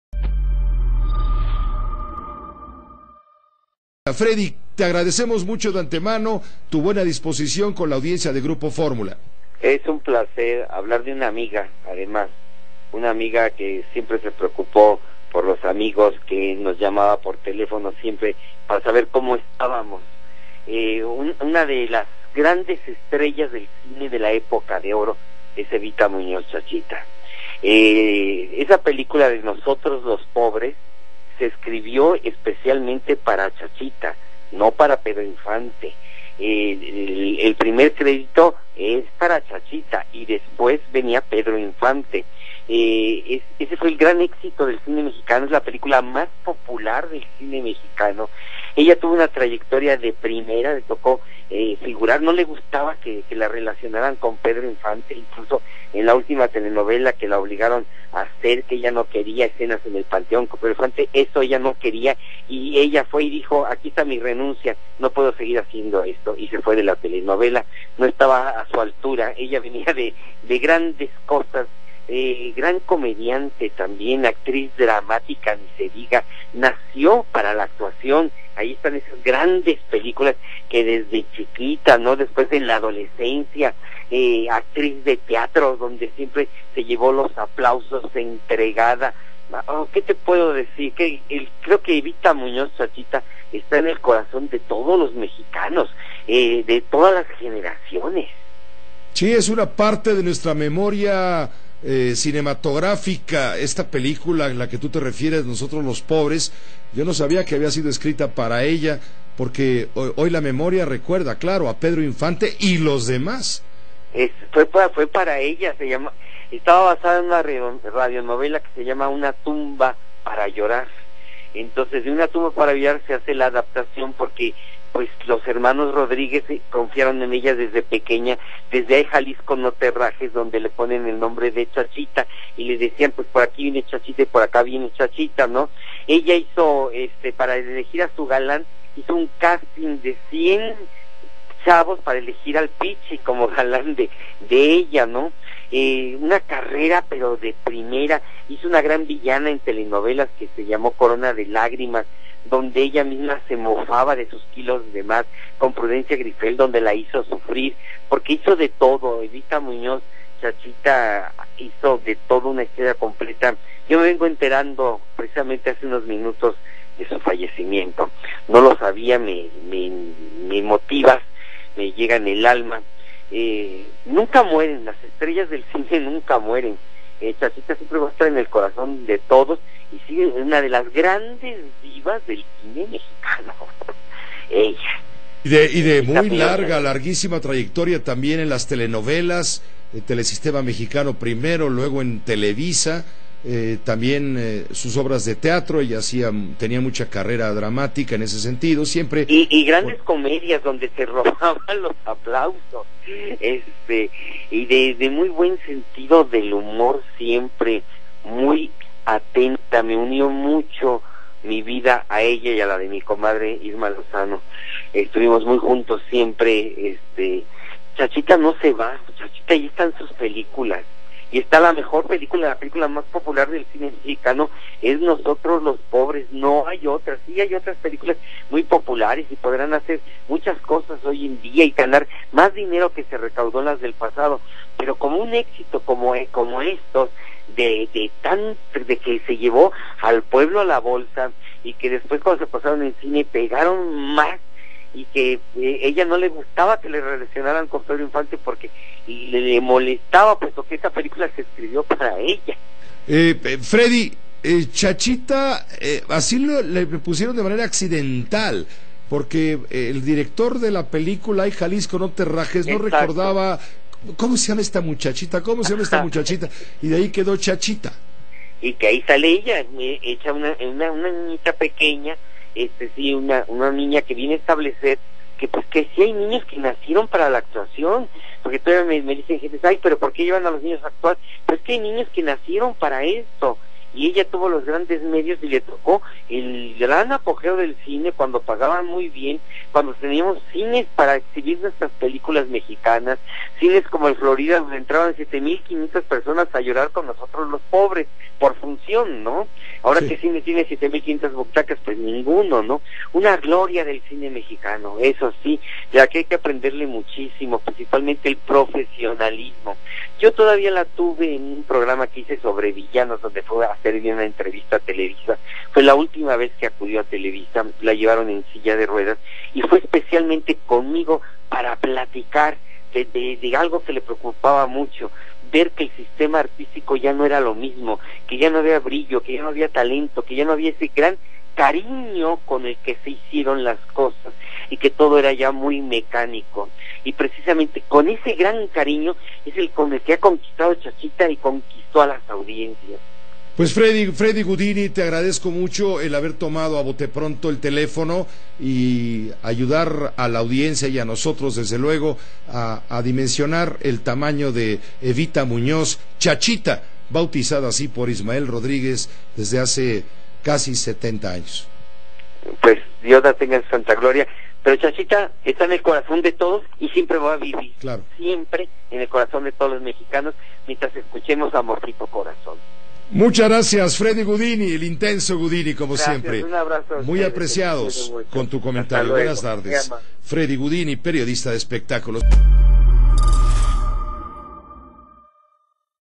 Con muerte de Chachita se pierde una de las grandes estrellas de la Época de Oro Gudini Con J.C.
Grupo Fórmula

El crítico de cine, Freddy Gudinni, resaltó que la película "Nosotros los pobres" estaba basada en una radionovela llamada "Una tumba para llorar", se escribió especialmente para "Chachita", no para Pedro Infante, el primer crédito fue el de Evita Muñoz. Esa cinta fue uno de los más grandes éxitos del cine mexicano, la más popular, ella tuvo una trayectoria de primera".